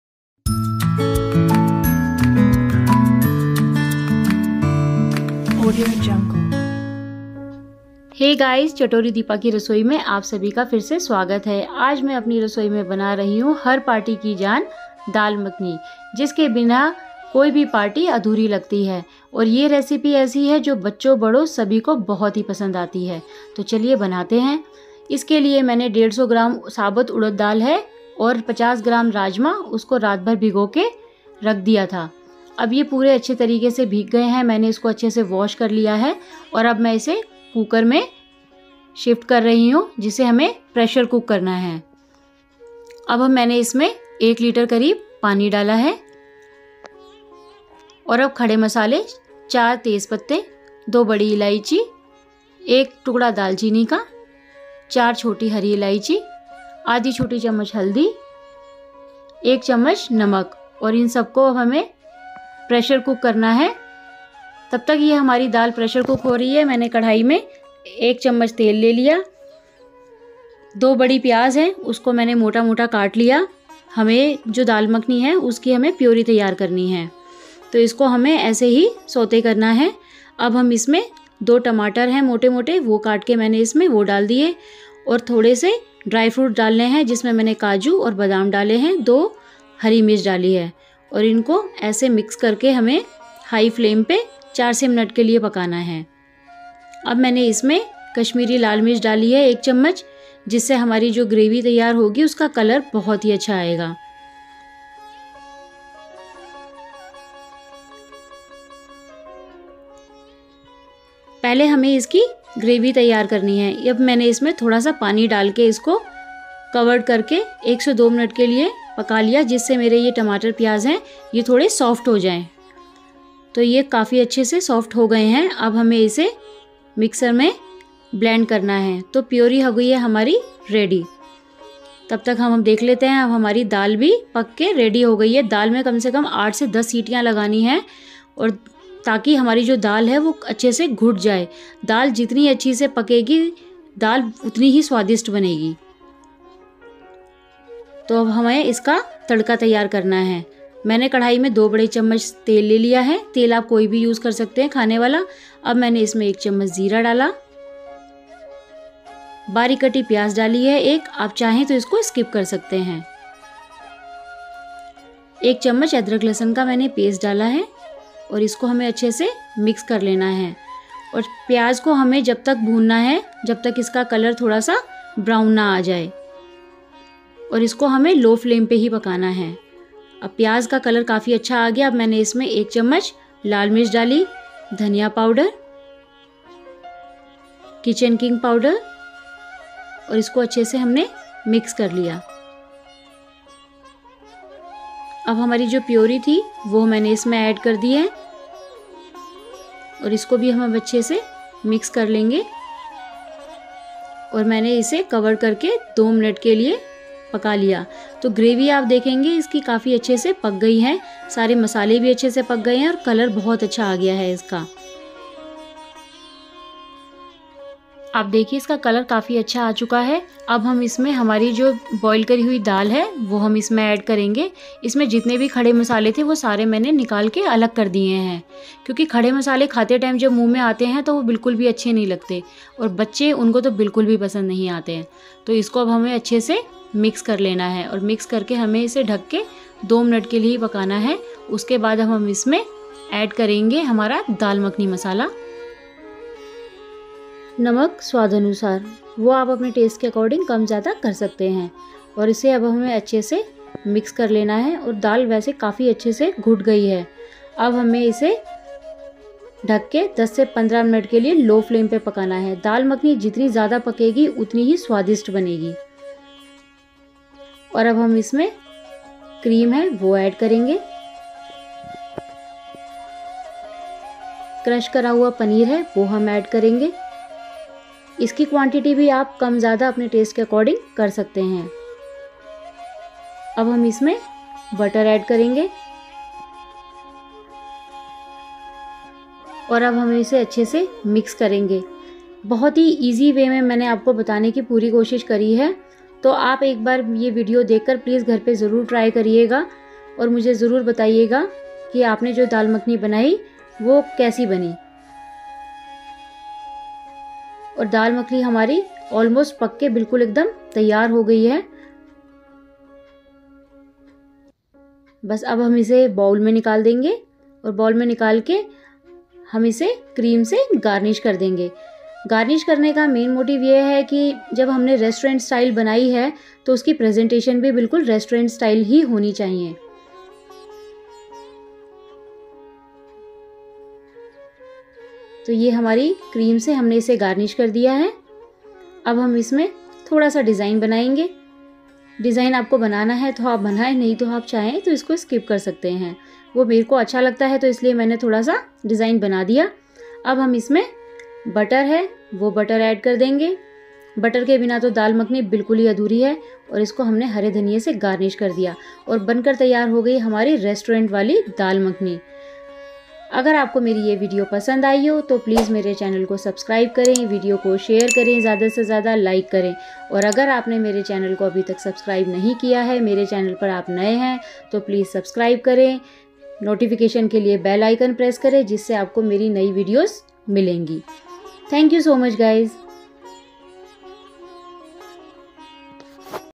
घर पर बनाये अब रेस्टोरेंट वाली दाल मखनी आसान तरीके से | Dal makhani recipe| How to make dal makhani
"Hello everyone! Namaste sabhi ko! Today we'll learn how to make the classic and flavorful Daal Makhni. Aaj hum sikhenge kaise banayein yeh mazedar Daal Makhni. This creamy and aromatic lentil dish is loved by all and it's perfect for any occasion. Is makhmali aur khushbu dar daal ko sabhi pasand karte hain aur yeh har mauke ke liye upyogi hai. So let's get started with the ingredients and step-by-step recipe. Toh chaliye, shuru karte hain samagri aur kadam-badam rasoi ki yatra. Happy cooking! Khushiyon bhara khana pakaye!"

Ingredients:

1 cup whole black lentils (urad dal)
1/4 cup red kidney beans (rajma)
1ltr water
4 Tez patta
2 Big cardamon
1 cinnamon
4 green cardamom

 For Puree:

2 tbsp oil
2 Onion finely chopped
2 Green chilli
2 tomatoes
1 tsp Red chilli powder

For Dal Tadka

2 tablespoon ghee or butter
1 small onion chopped
1 tablespoon ginger-garlic paste
1 teaspoon cumin seeds
1/2 teaspoon turmeric powder
1 teaspoon red chili powder (adjust to taste)
1 tablespoon coriander powder
1 teaspoon garam masala ( Kitchen King)
1teaspoon Dal makhni masala
1/4 cup fresh cream
Salt to taste
Crushed paneer
2 tablespoons chopped coriander leaves for garnish